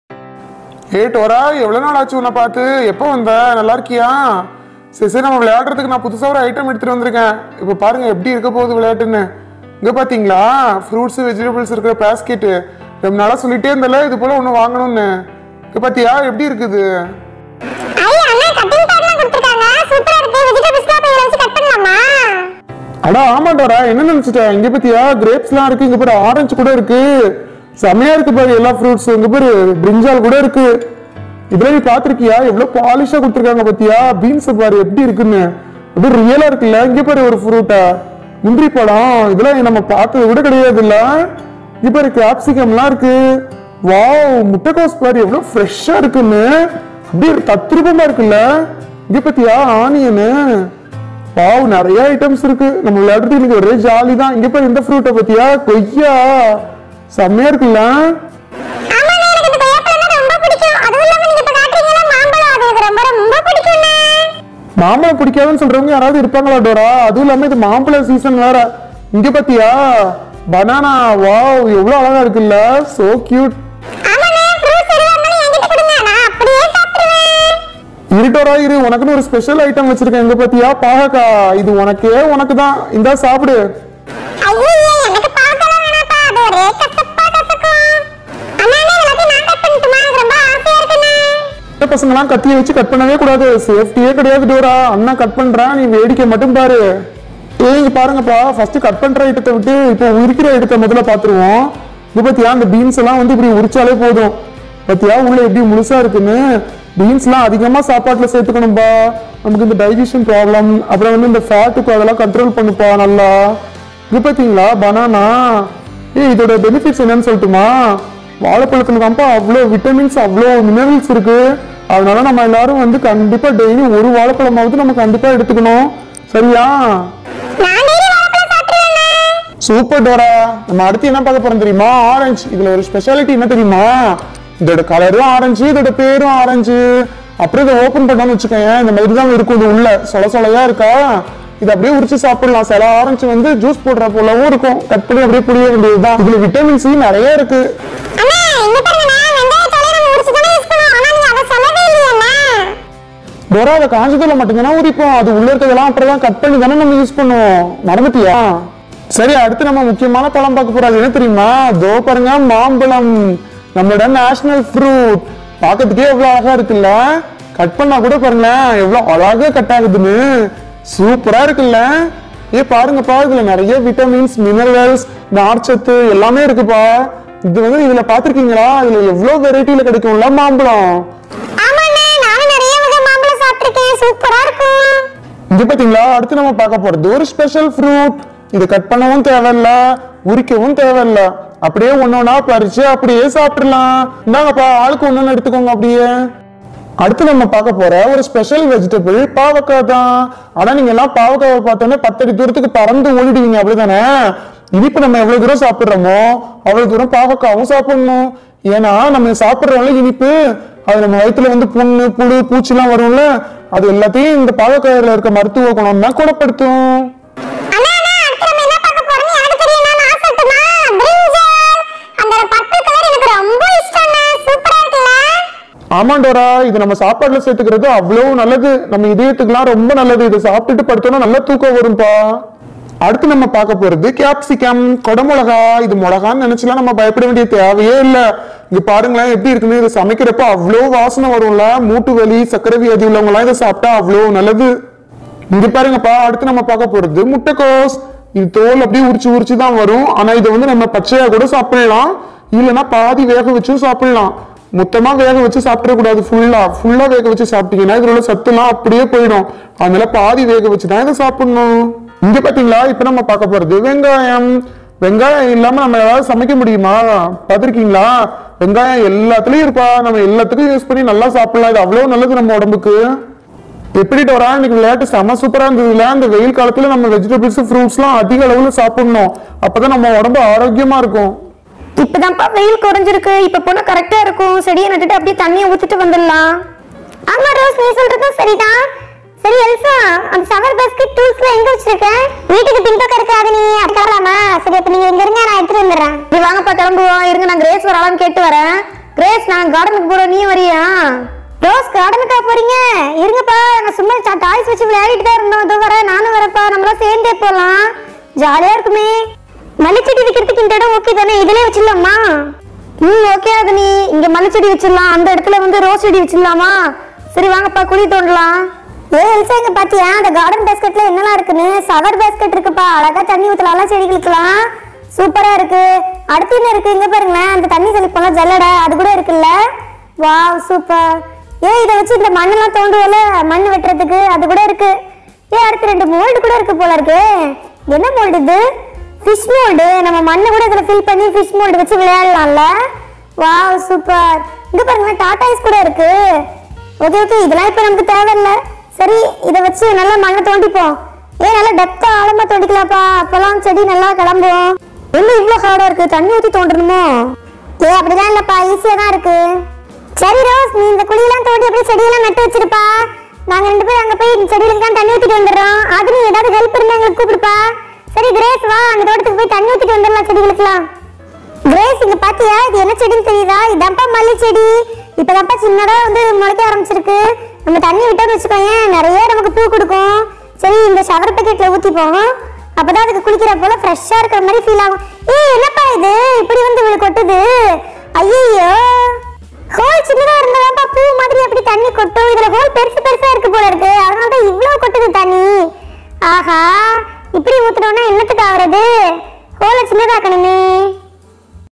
சுட்டி கலாட்டா 163 "barbie doll daily routine games in indian village" | barbie story time show
Story of this barbie show:
  In this show barbies playing fruits and vegetables cutting games specially they educate their uses.. girls team they engaged in garden works. its very interesting and useful video for our kids.. watch it video fully and if you like give your support 🙏.

We are always happy to hear from you! Please share your feedback in comment box..